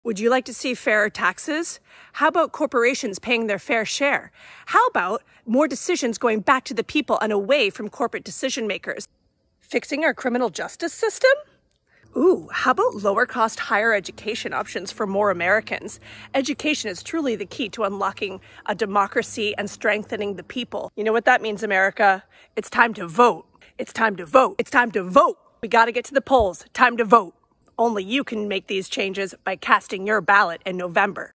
Vote Tuesday, November 8! Be there!
america , you heard me! It’s time to vote! registertovote or apply for an absenteeballot. Or see you in person at she polls on Tuesday, November 8! Let's gooooooo!